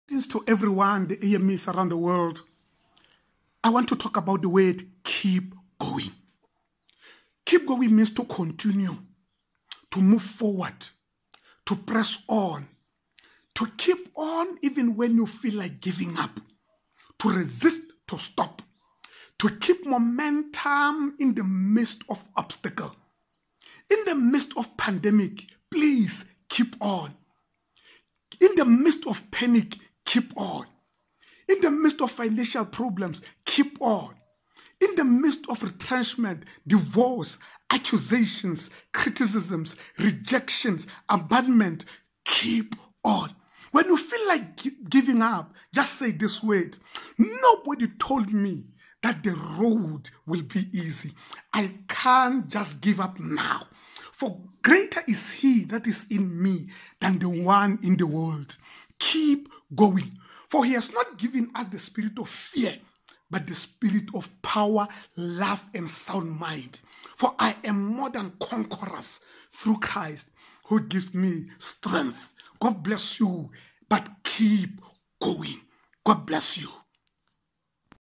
Motivational Minute Monday 6/14/2020 with Rev.Innocent Monare Presiding Elder of Aliwal North
MotivationalMinuteMonday with Rev. Innocent Monare, Presiding Elder of Aliwal North, Aliwal, South Africa, 15th District

“God Our Father, Christ Our Redeemer, the Holy Spirit Our Comforter, Humankind Our Family.”
African Methodist Episcopal Church
1720 Scovel St